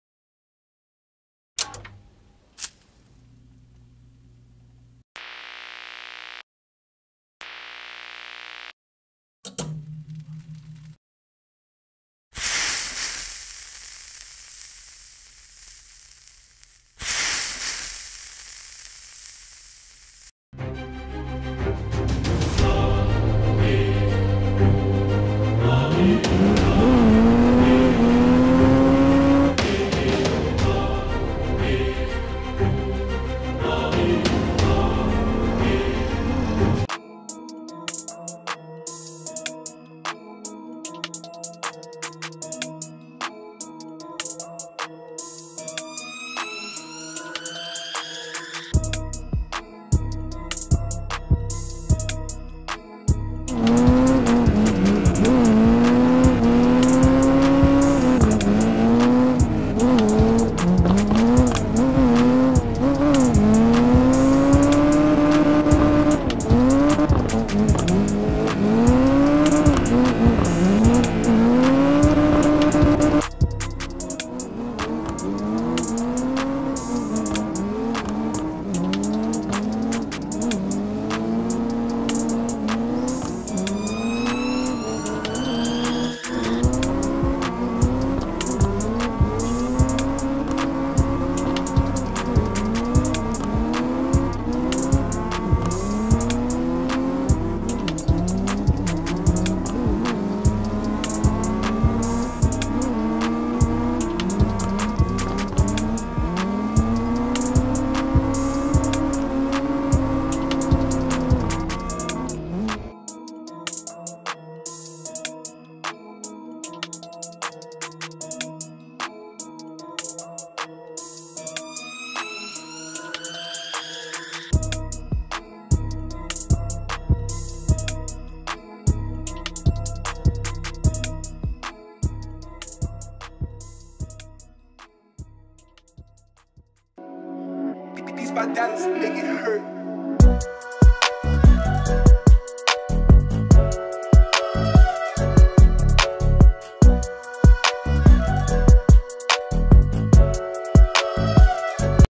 💥 TEAM TOPLINE PDC 2021 TEST AND TUNE 💥 // Quick Highlight Edit!!
HEY EVERYONE!

Just a short edit recapping some of my highlights from the teams test and tune day for PDC 2021! Thanks for stopping by and showing your support!

XBL: TTL Kouki

Nissan Silvia S15
- 750hp 9,200rpm SR22
- HGT Dogbox Gear ratios w/ 4.30 F/D
- 245/265R18 120adhesion tires
- GRF Tuning suspension and alignment
- URAS Fenders/Origin Labo Kit

Wheel Setup and Settings
- Thrustmaster TMX Pro
- T3PA Pedals with Ricmotech Springs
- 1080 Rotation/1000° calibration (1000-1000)
- NO ASSIST
- 0.6 FFB/1.0 Linearity/0.0 Deadzone